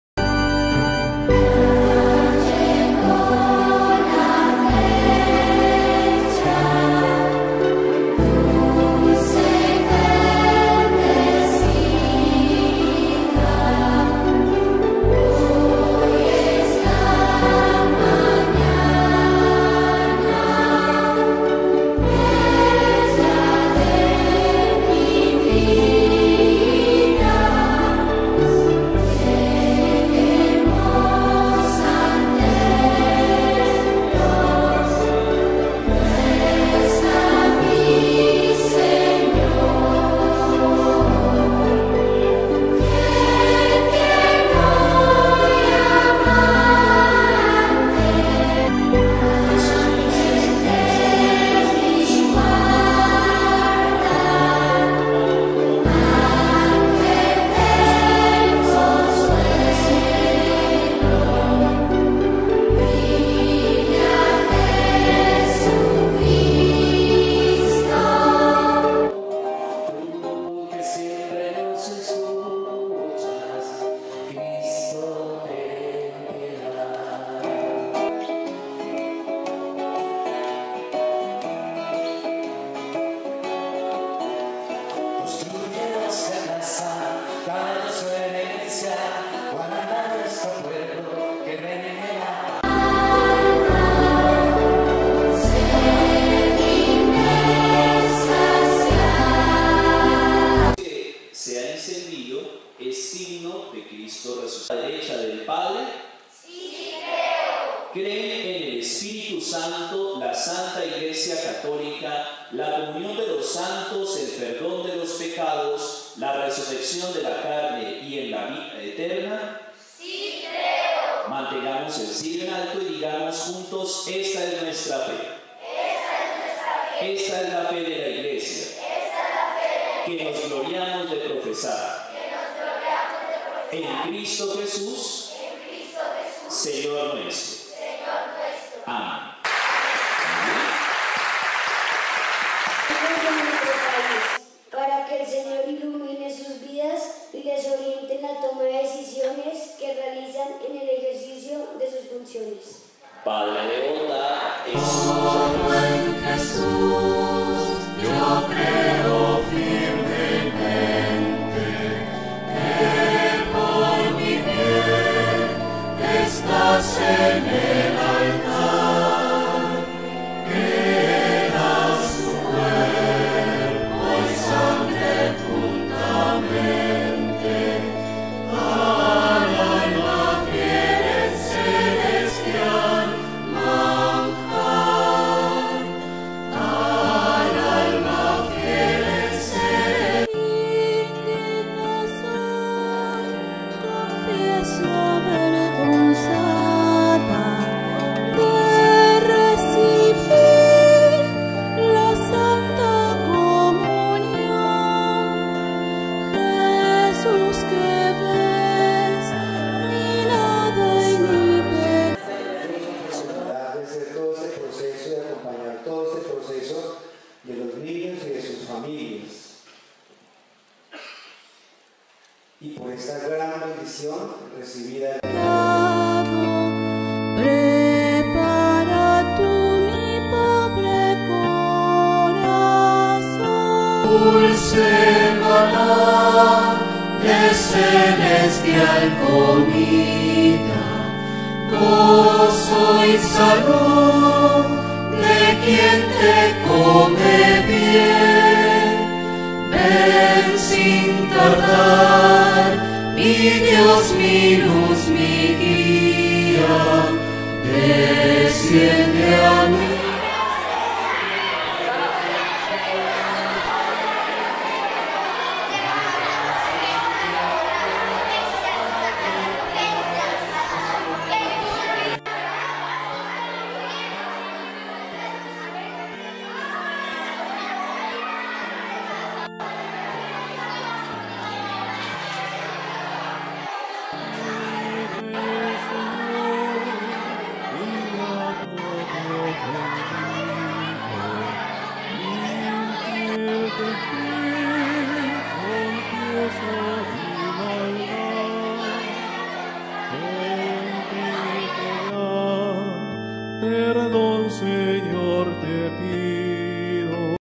Primeras Comuniones 2018 - Col. CHAMPAGNAT - Bogotá
PRIMERAS COMUNIONES 2018
EN EL COLEGIO CHAMPAGNAT DE BOGOTÁ

El pasado 18 de agosto se celebraron en el colegio Champagnat de Bogotá las primeras comuniones que llevan como nombre: Eucaristía Sacramento que fortalece mi vida en familia.

El proceso se llevó a cabo en cabeza del Coordinador de Evangelización Yhon Jairo Peña, el docente Jeisson Medina y la catequista Sandra Morales.

La celebración fue presidida por el Pbro. Onías Ossa Coronado, sacerdote de la Arquidiócesis de Bogotá 62 niños recibieron por primera vez a Jesucristo Nuestro Señor.